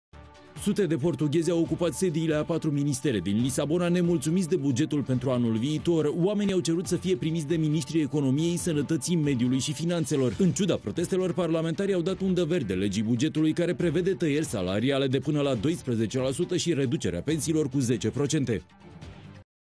Sediile mai multor ministere din Lisabona, ocupate de sute de protestatari
Sute de portughezi au ocupat sediile a patru ministere din Lisabona, nemulţumiţi de bugetul pentru anul viitor.